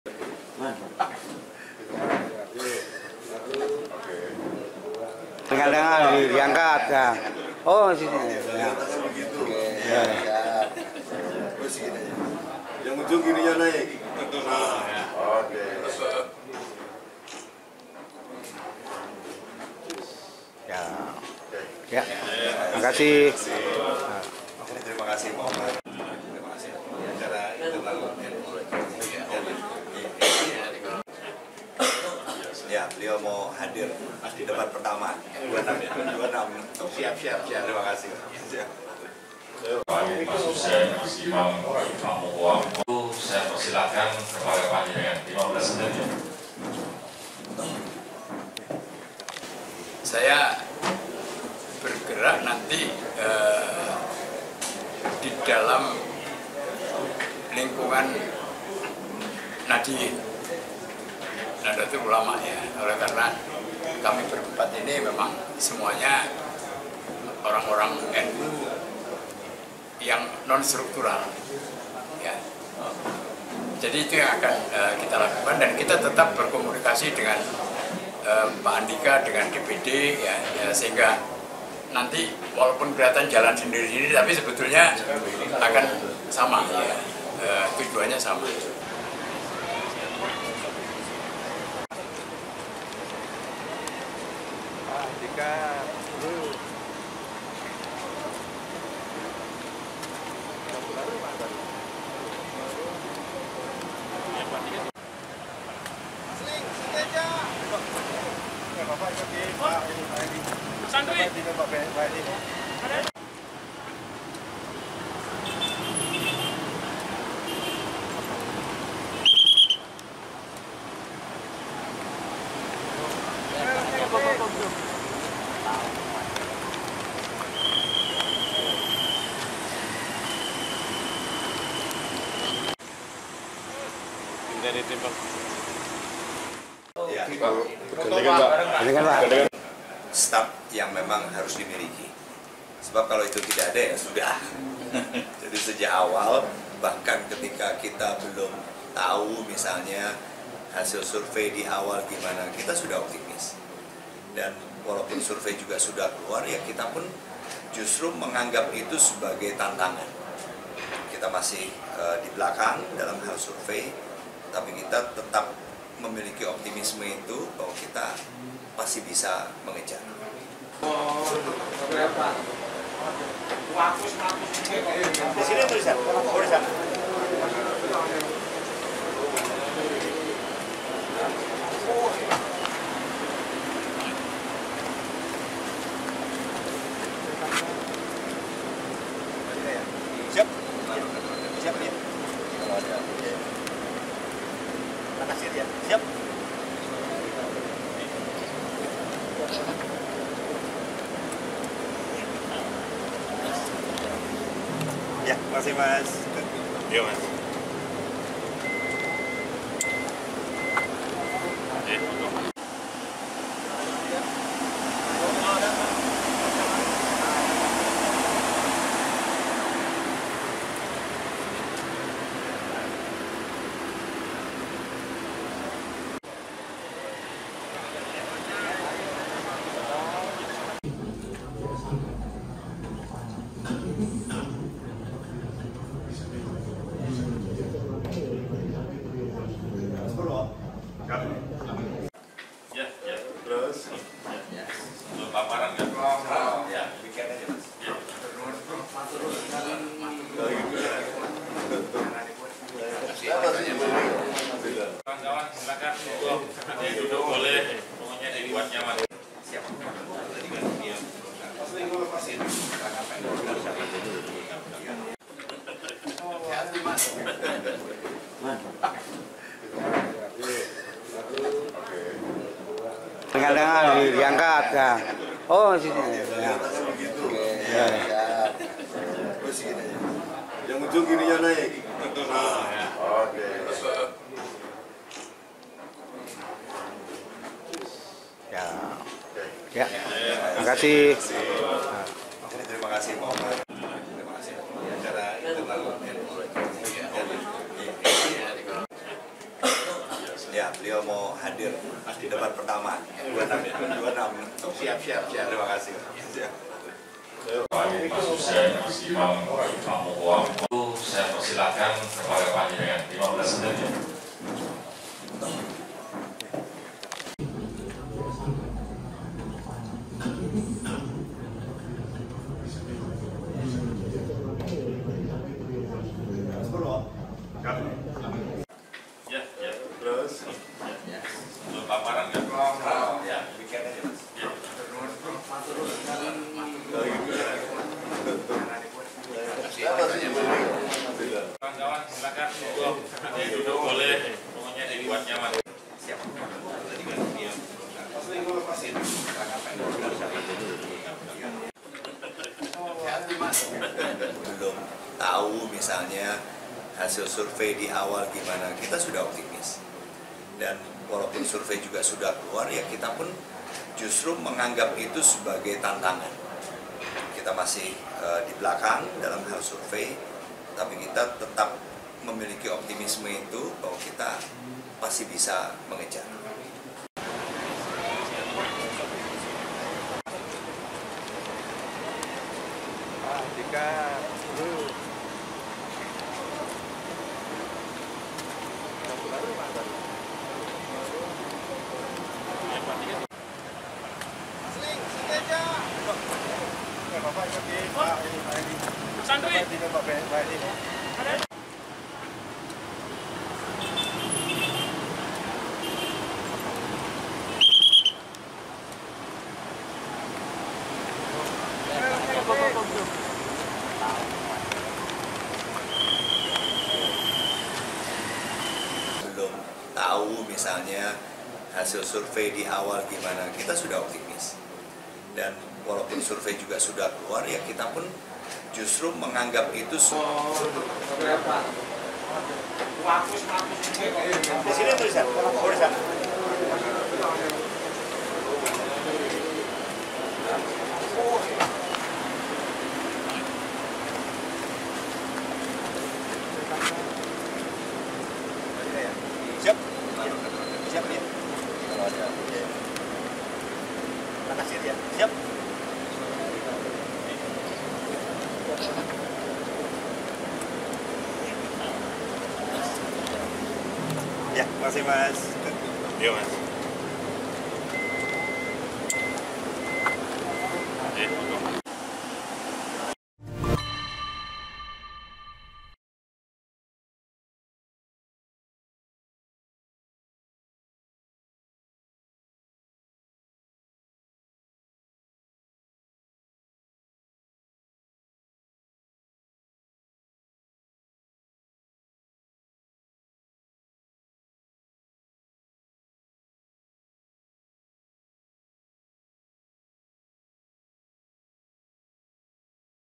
Andika Perkasa-Hendrar Prihadi Gelar Rapat Tim Pemenangan untuk Pilkada Jateng 2024
Rapat tim pemenangan yang berlangsung di posko kemenangan Jateng Perkasa ini merupakan kelanjutan dari rapat sebelumnya yang diadakan di kantor DPD PDI Perjuangan Jawa Tengah. Pelaksanaan rapat yang tertutup ini dihadiri oleh Kyai Haji Umar Wahid, yang juga merupakan adik Gus Dur.

Setelah rapat, Kyai Haji Umar Wahid menegaskan bahwa sebagai bagian dari Nahdlatul Ulama, mereka akan fokus pada kalangan Nahdliyin non-struktural untuk mendukung pasangan Andika Perkasa-Hendrar Prihadi dalam Pilkada Jawa Tengah 2024.

Ia juga menekankan pentingnya koordinasi dengan DPD PDI Perjuangan Jawa Tengah dan pasangan calon untuk mencapai kemenangan. Calon Gubernur Jawa Tengah Andika Perkasa, mengungkapkan optimisme untuk meraih kemenangan setelah melewati masa sosialisasi dan awal kampanye. Meskipun hasil survei saat ini belum menunjukkan keunggulan, Andika menjadikannya sebagai tantangan untuk terus mengejar suara. PWHY